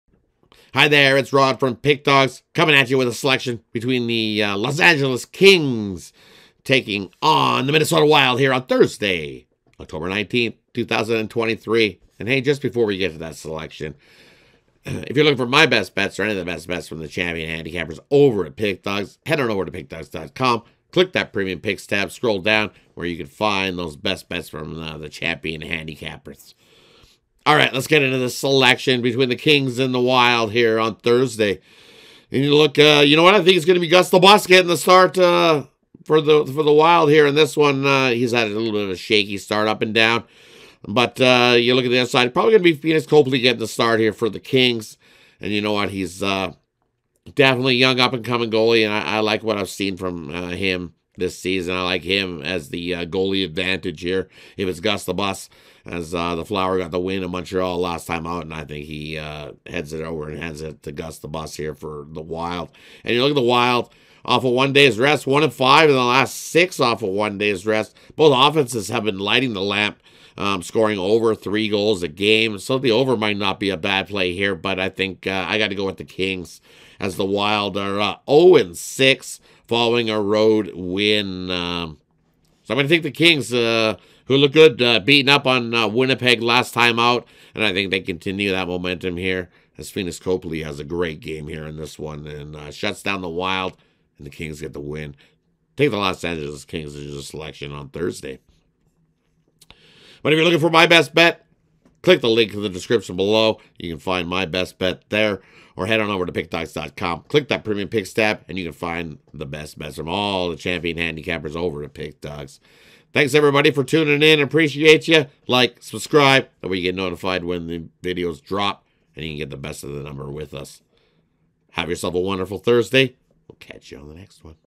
Los Angeles Kings vs. Minnesota Wild 10/19/23 NHL Free Pick | NHL Betting Tips
The Minnesota Wild face the Los Angeles Kings in Thursday's NHL action. Get The Puckdawgz' NHL Pick and Prediction for this game.